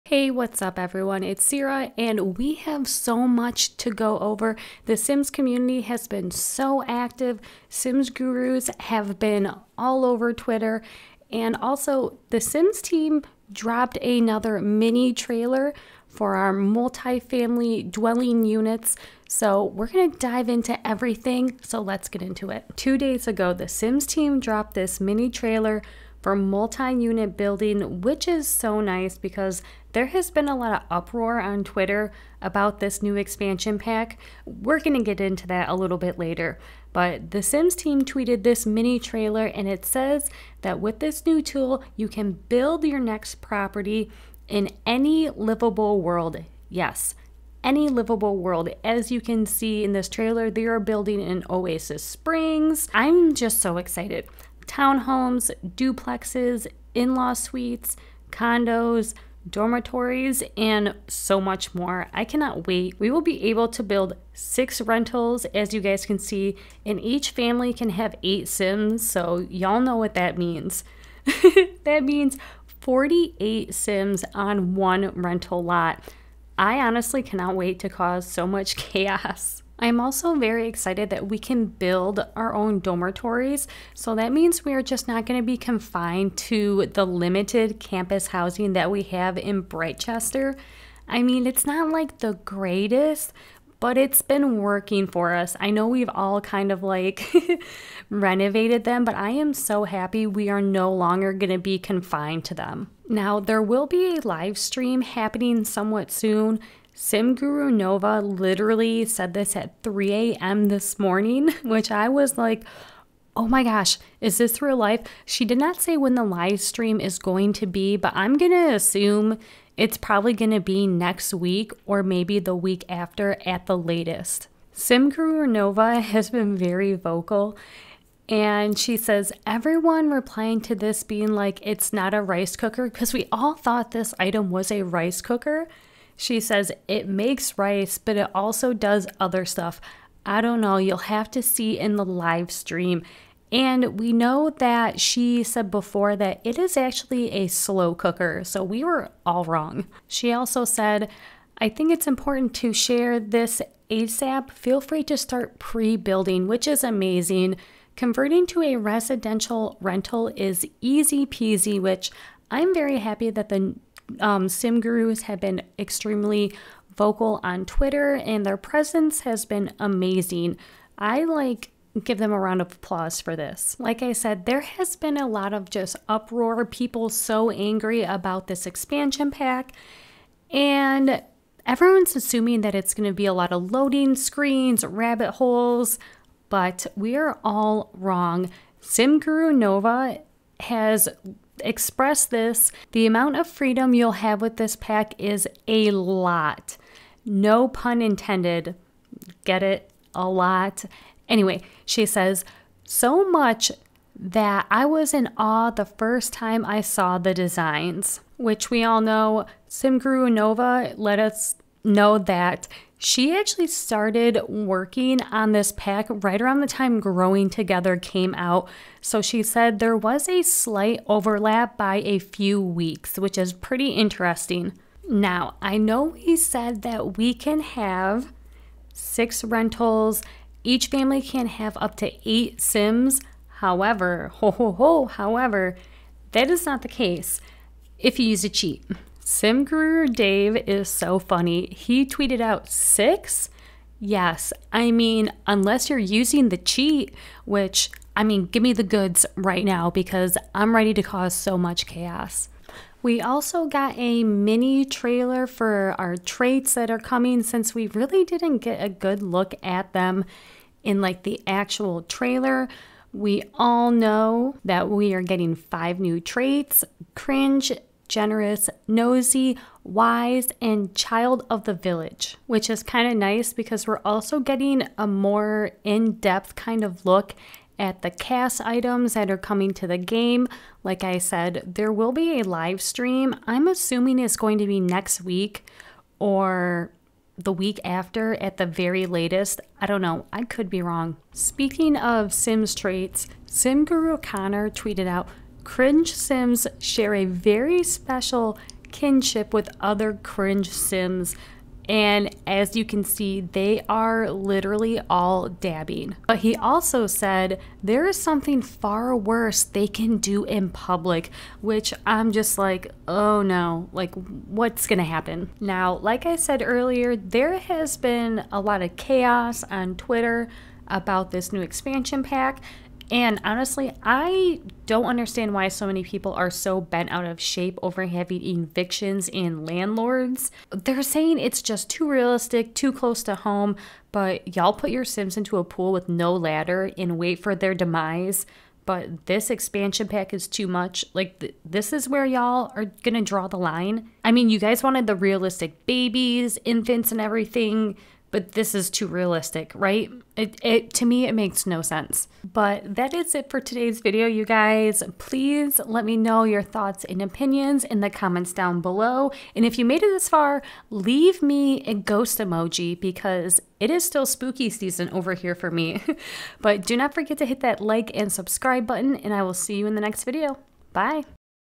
Sims 4 For Rent multi-unit tool, live stream announcement and more!
Sim Guru's have been busy the past few days!
Sims community is not happy about new EP?!?

Sims ID: cearaplayssims